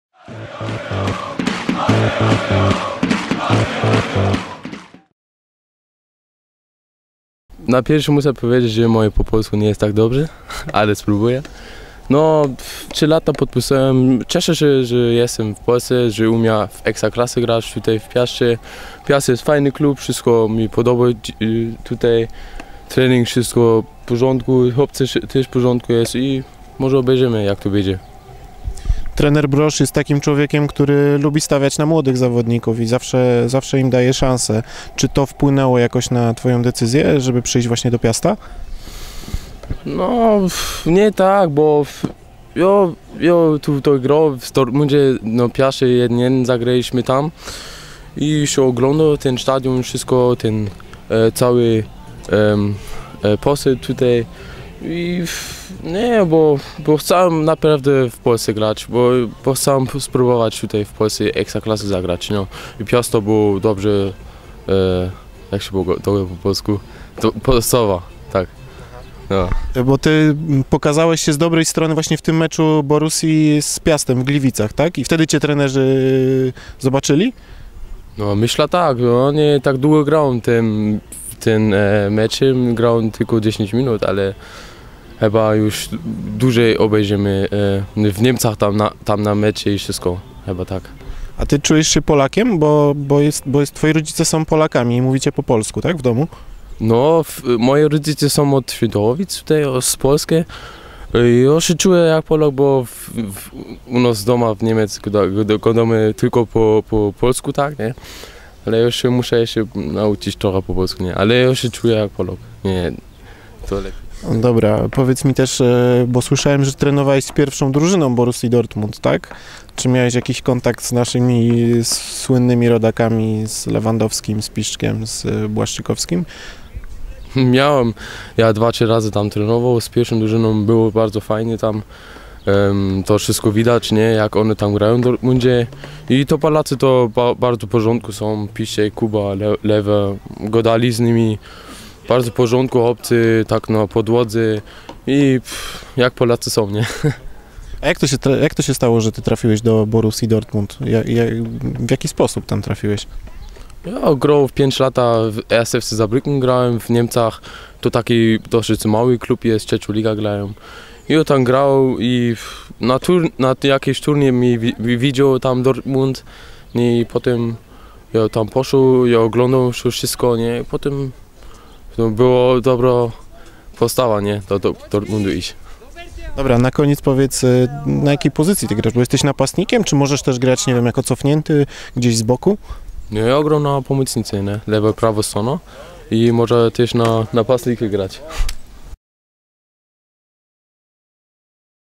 Patryk Dytko: Piast Gliwice to bardzo fajny klub
Kolejnym z letnich nabytków Piasta jest Patryk Dytko, który do Gliwic trafił z Borussii Dortmund. Pomocnik w wywiadzie z klubową telewizją opowiada dlaczego zdecydował się na grę w niebiesko-czerwonych barwach.